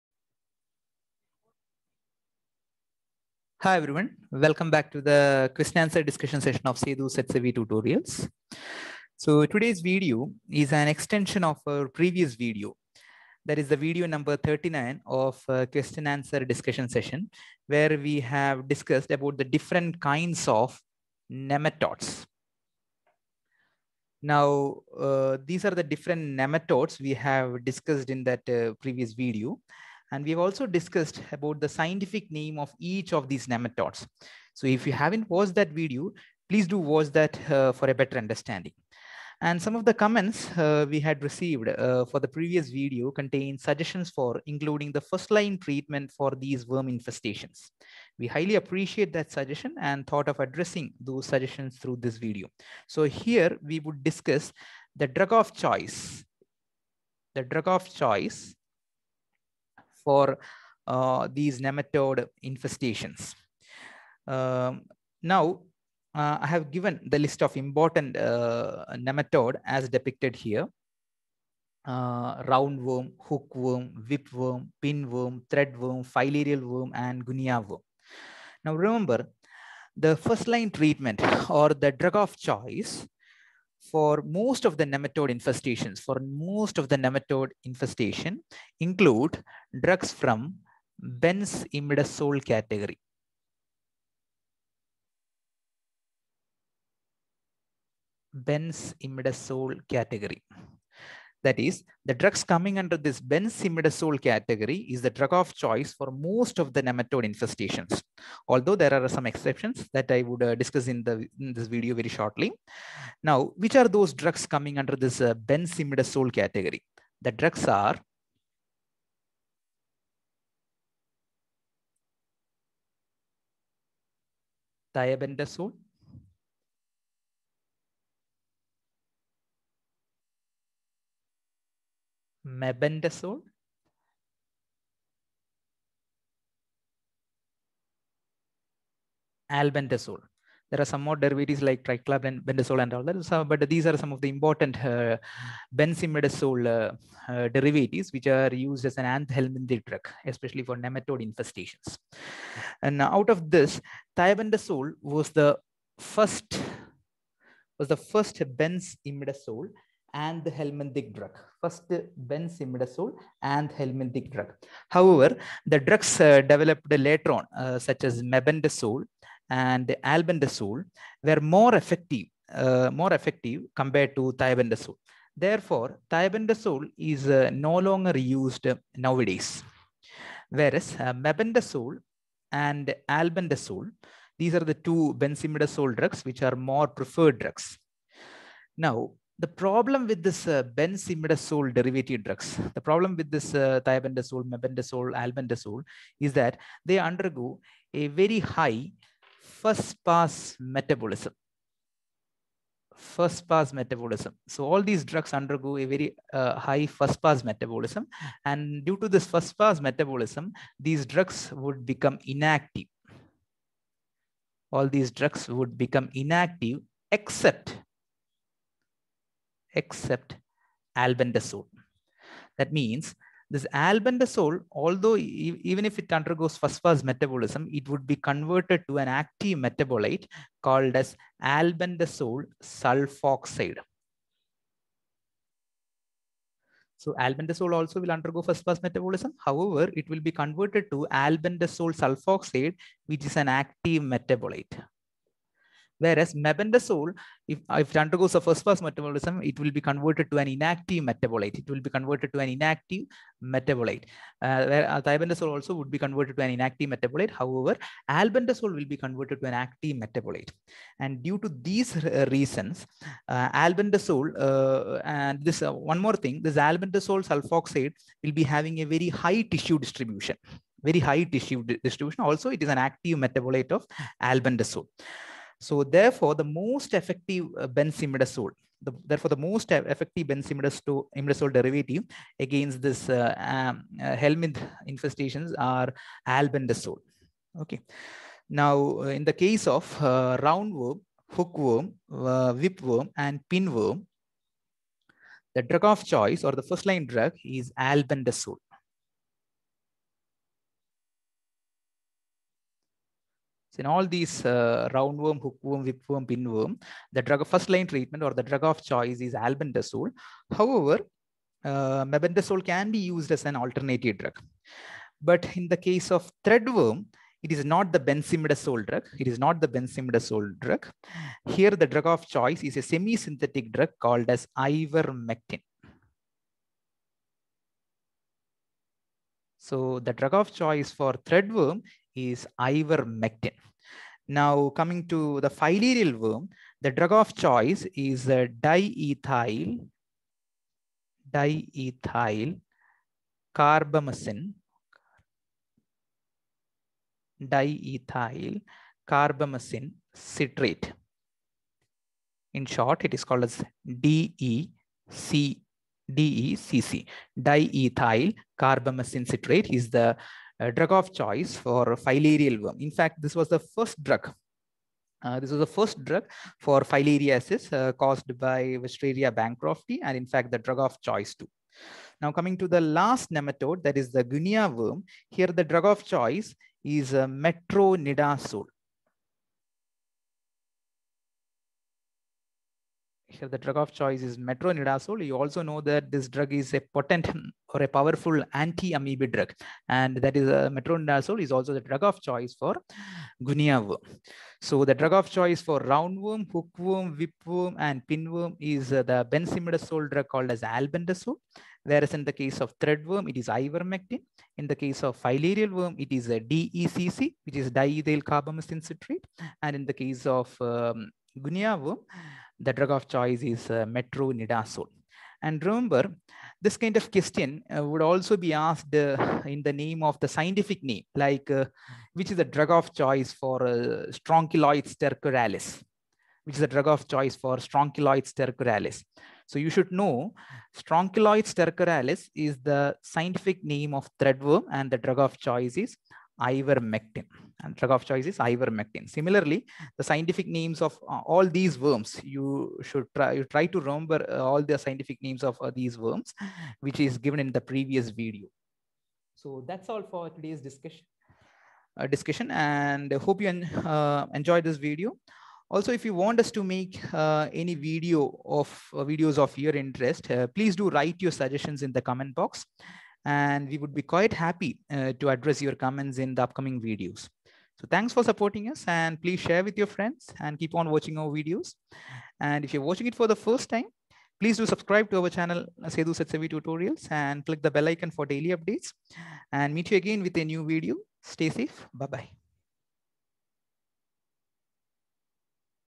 Pharmacy Question and Answer discussion: Q40; Drug of Choice for Nematodes (Helminths/Worms)
This video is regarding the First-line treatment or Drug of Choice (DOC) for various nematodes coming under Helminths or Parasitic worms.

This video will be highly beneficial for Pharmacy (B.Pharm., Pharm.D, D.Pharm), Nursing, and Medical professionals.
Highly useful for GPAT, NIPER, PHARMACIST,, DRUG INSPECTOR, ANALYST, NURSING, MEDICAL, COMPETITIVE EXAMS